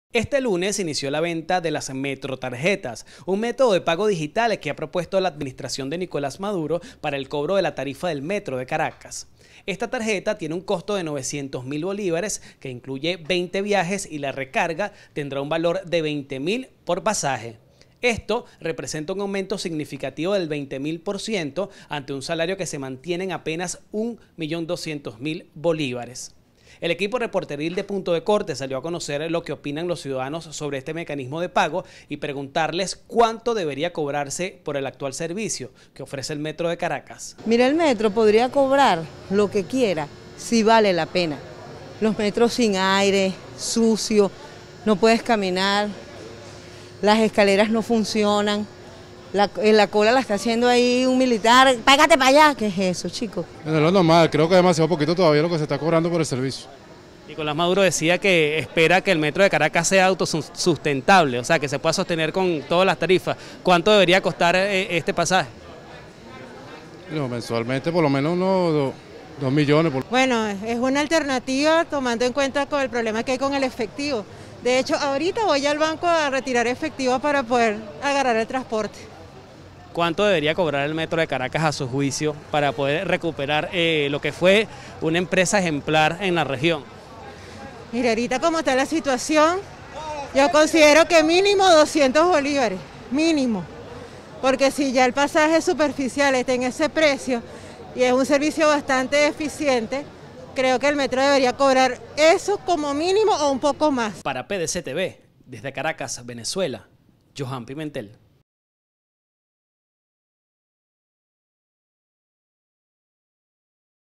¿La nueva tarifa del Metro de Caracas podrá mejorar su servicio?
[Si no logras abrir el link, descarga un VPN en la Store de tu sistema operativo y vence la censura]